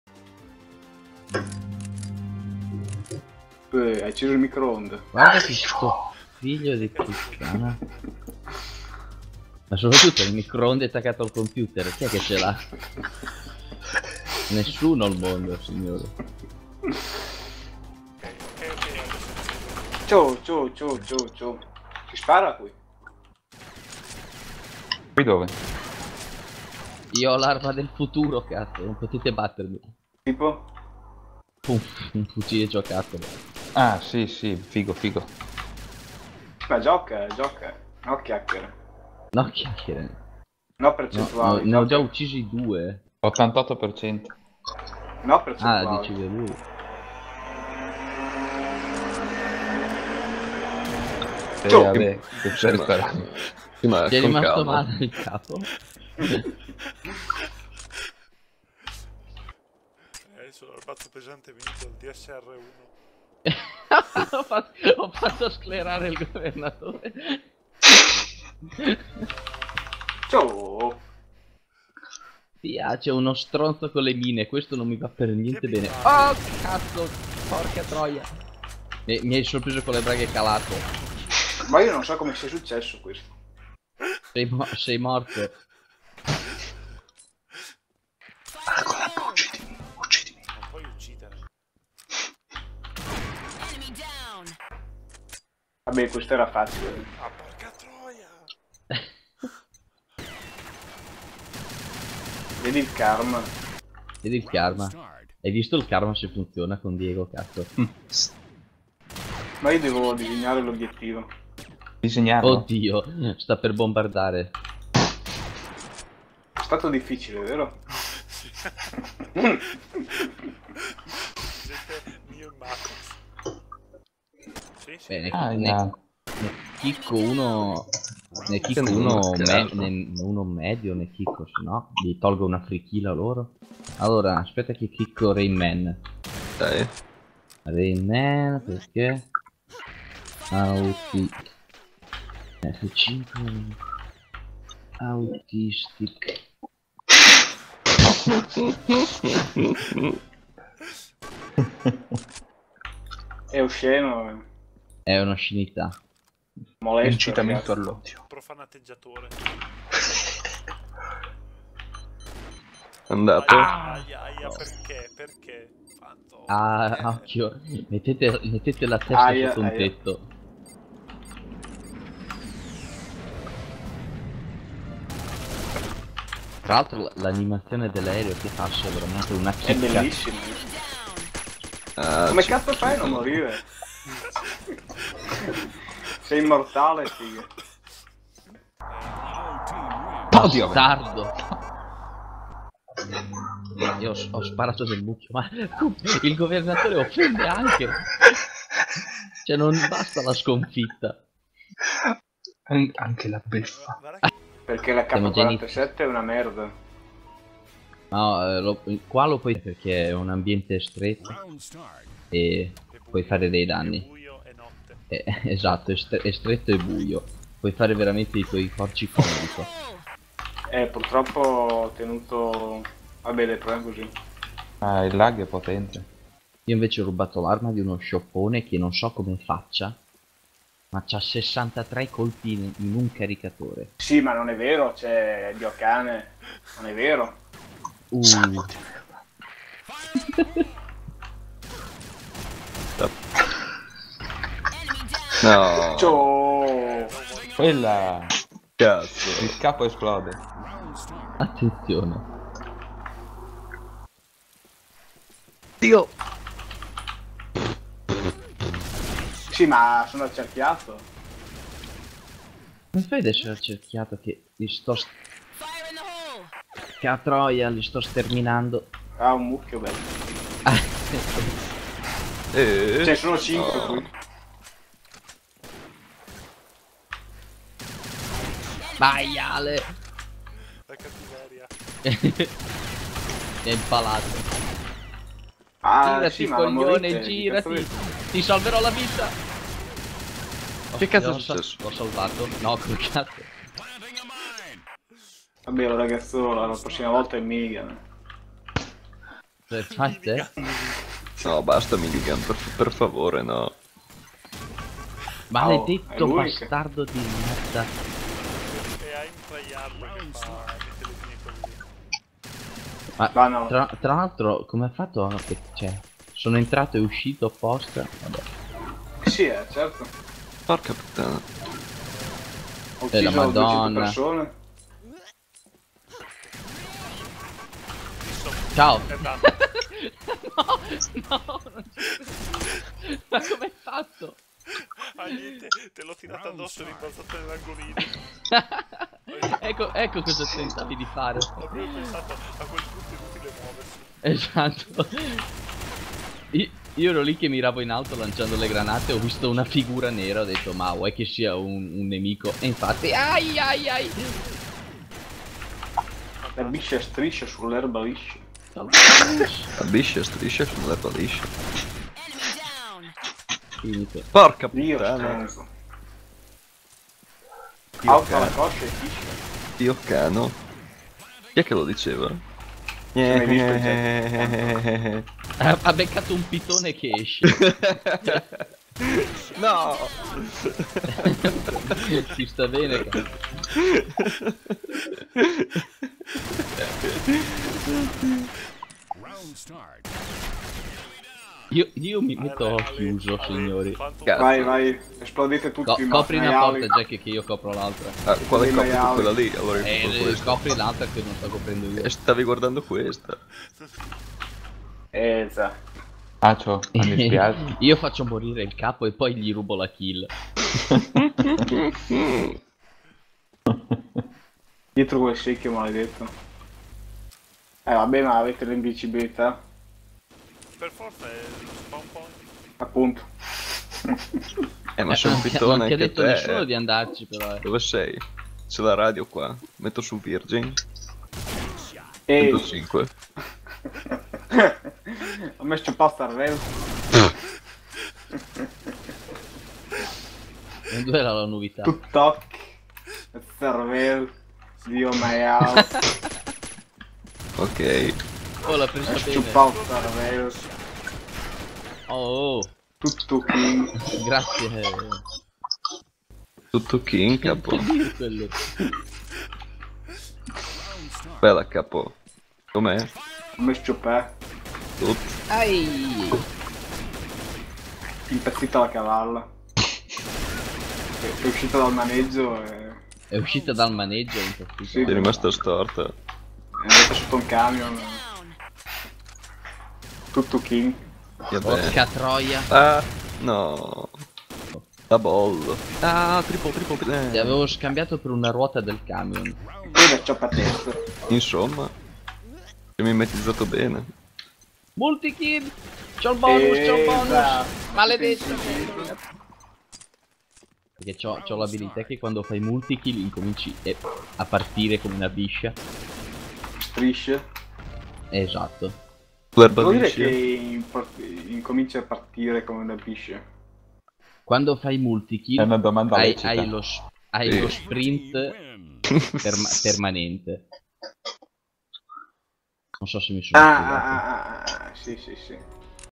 Ignoranza Time 16 - Al capo esplode il cervello
Nel sabato sera che non ti aspetti il capo ha problemi di consistenza del cranio